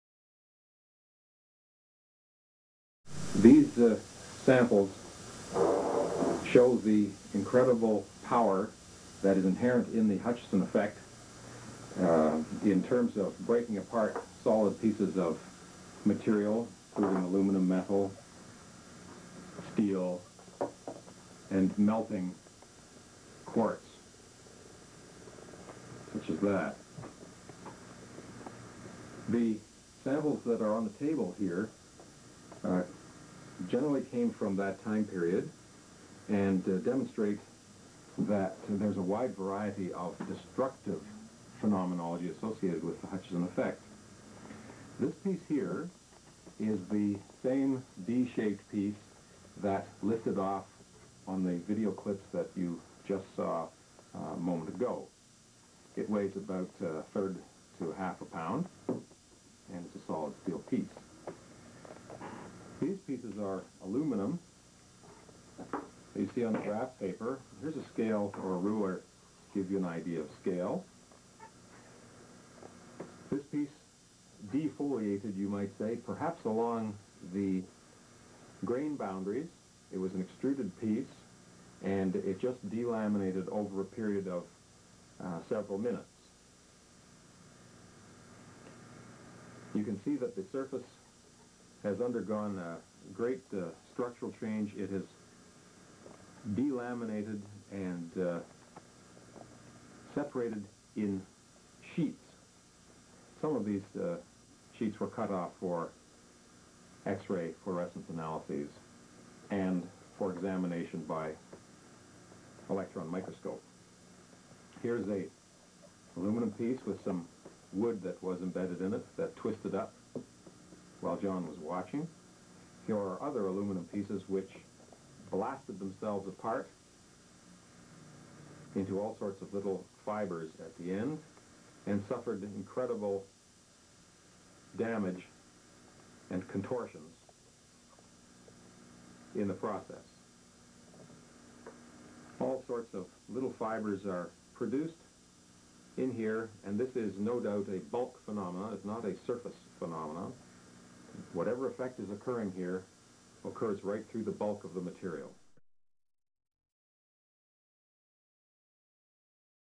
The Hutchison Effect: George Hathaway on Metal Jellification
- Dr. George Hathaway demonstrates how the Hutchison Effect alters the properties of various materials and induces jellification in metals in this 1980's footage taken from a VHS overview of the Hutchison Effect by Hathaway.

As John Hutchison's lab-partner in early research, Dr. George Hathaway participated extensively in researching & filming the Hutchison Effect experiments, and in this video he describes a number of interesting samples that he participated in testing.

The Hutchison Effect includes a number of strange effects, but some of the most pronounced are the changes in materials induced by the various resonant fields that occur in the Hutchison Effect. Steel, aluminum, brass, and other metals are literally turned into a jelly-like substance that often mixes with other materials like plastic and wood - when the fields are turned off, the metals re-harden with their original properties, but typically have a distorted molecular structure in the process.

Later research by George Hathaway demonstrated additional data: first, that the Hutchison Effect in materials typically begins at the center of the material and works it's way towards the ends, and second, that impurities in all metal samples are worked through the molecular structure of jellified metals and "pushed" towards the ends of the sample, where they were found in concentrated amounts.